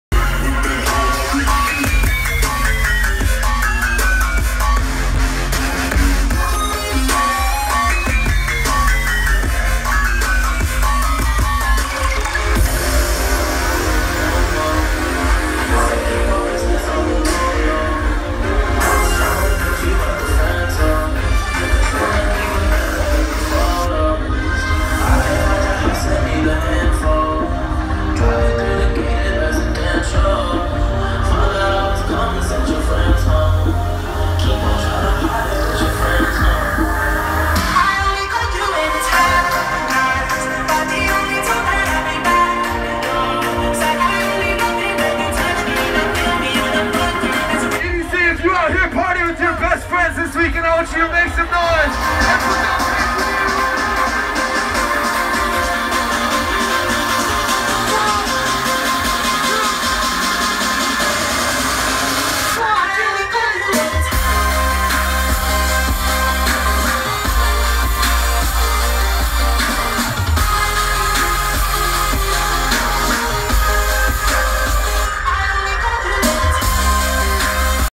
NGHTMRE @ EDC Orlando 2016 (Clip 1)
Here's a clip about two-thirds of they way into NGHTMRE's set at EDC Orlando 2016.  He performed twice (this was recorded during his Circuit Grounds appearance) on November 4 (Day 1).  The drop master did a fantastic job for what I believe was his first EDC appearance!  To see all my content from this set, visit the post @